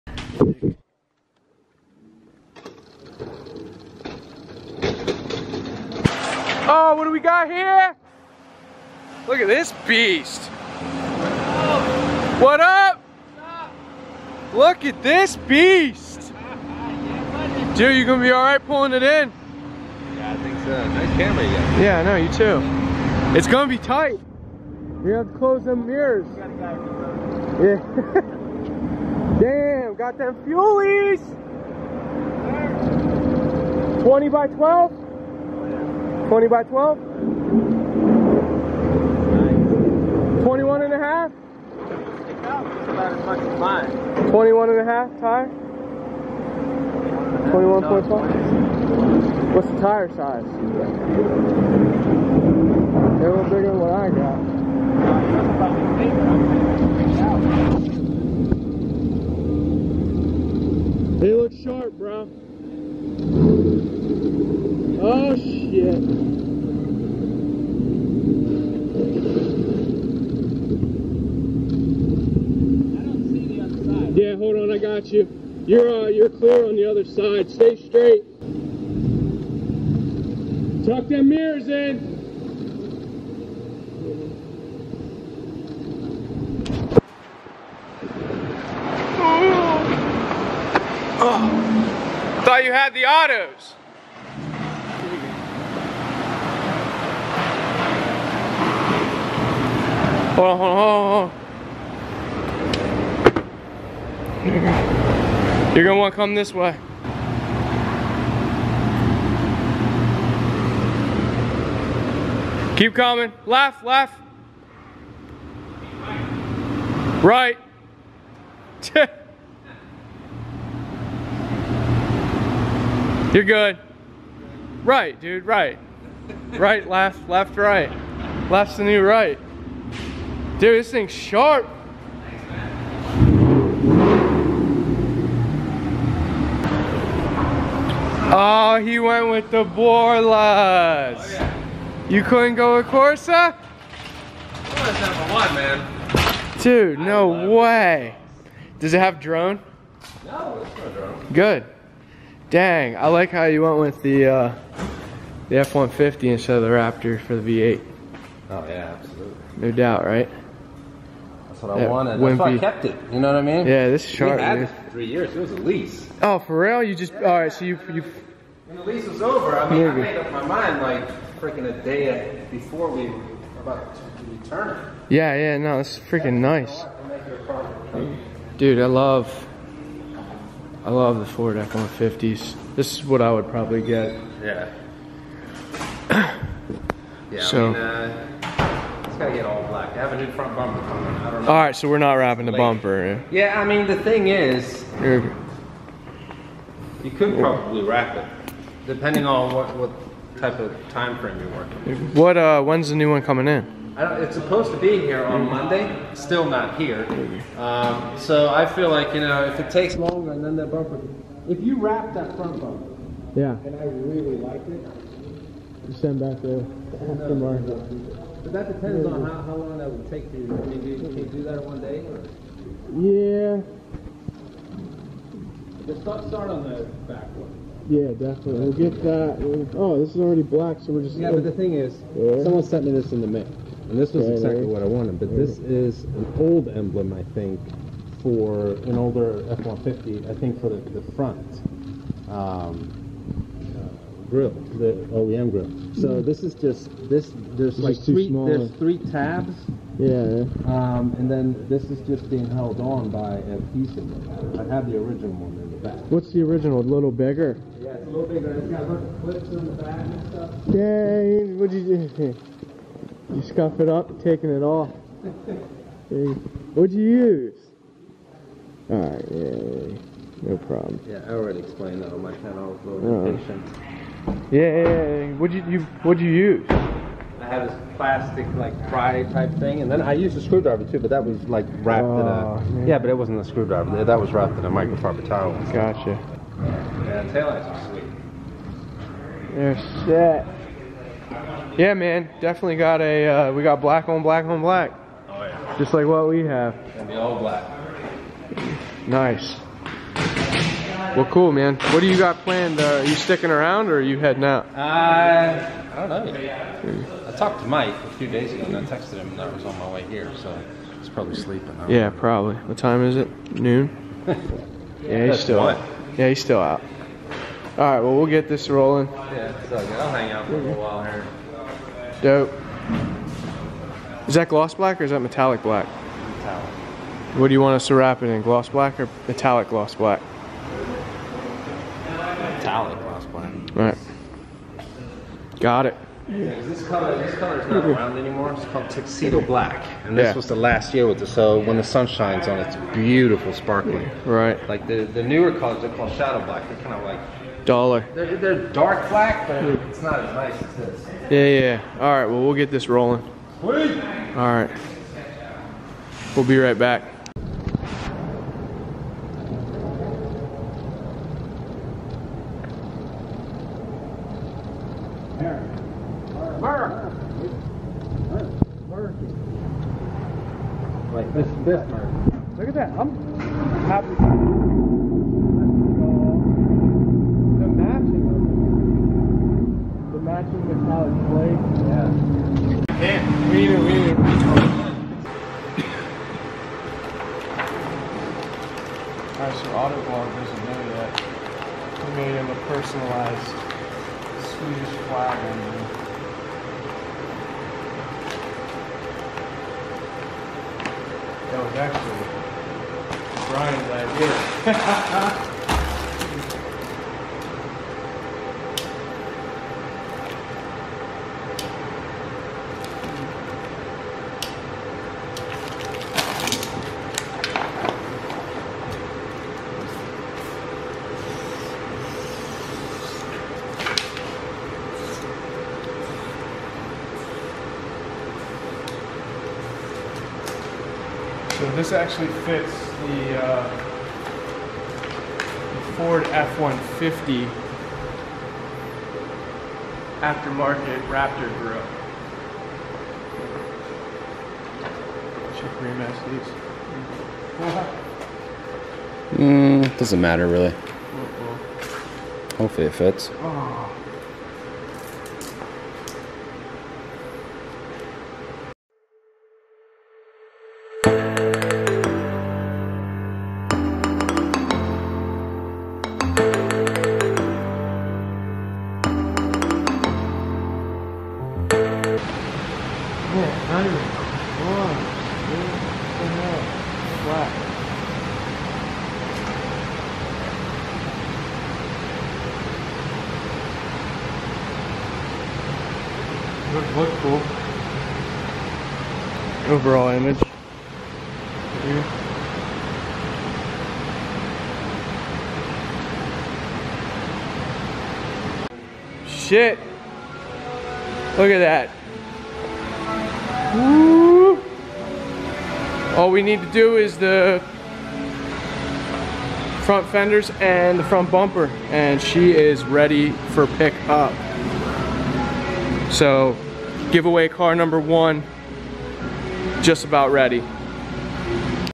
Auto Vlog's F-150 Gets A Makeover!
Thanks to Auto Vlog for bringing out his F-150 for us to have fun with! Make sure to check out Auto Vlog!